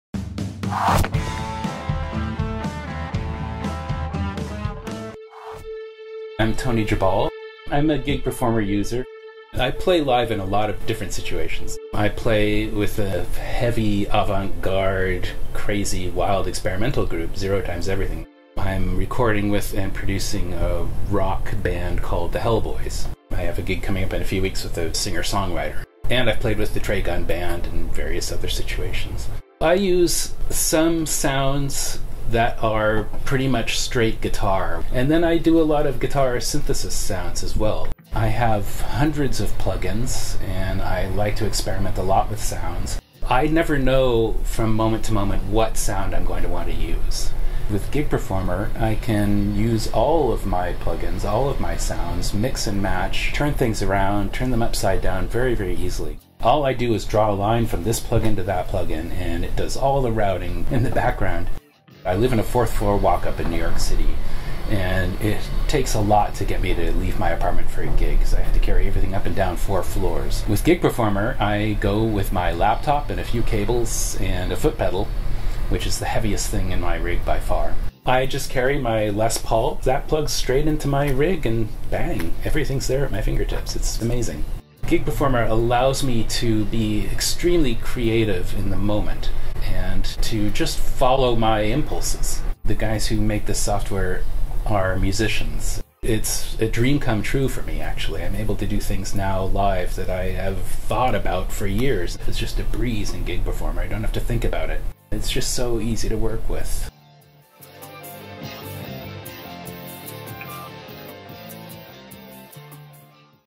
Gig Performer Multiplies Tony Geballe’s Guitar Times Everything
Composer, guitarist, sound designer, producer, and mixer Tony Geballe has spent years working closely with musicians from King Crimson and Peter Gabriel’s bands, and performed in settings ranging from solo acoustic 12-string guitar to “progressive darkwave.”

Geballe leverages a variety of processor and synthesizer plugins to generate a broad sonic palette, and needs extensive control of the entire setup at his fingertips—or, more accurately, at his feet. Making it all work requires plenty of footpedals and switches in performance, and considerable time spent configuring his setup in advance. Geballe centers the whole system on Deskew Technologies’ Gig Performer software.